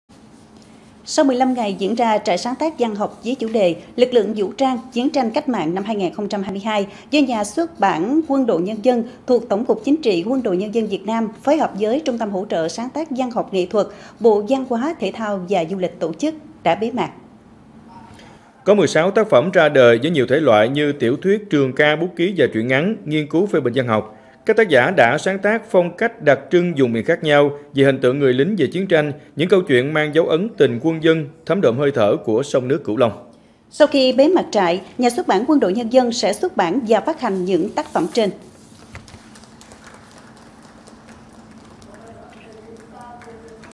Bế mạc trại sáng tác văn học “Lực lượng vũ trang - Chiến tranh cách mạng” | Cần Thơ TV
Sau 15 ngày diễn ra, trại sáng tác văn học chủ đề "Lực lượng vũ trang - Chiến tranh cách mạng""năm 2022, do Nhà xuất bản Quân đội nhân dân thuộc Tổng cục chính trị Quân đội Nhân dân Việt Nam phối hợp Trung tâm Hỗ trợ Sáng tác Văn học nghệ thuật - Bộ Văn hóa, Thể thao và Du lịch tổ chức đã bế mạc.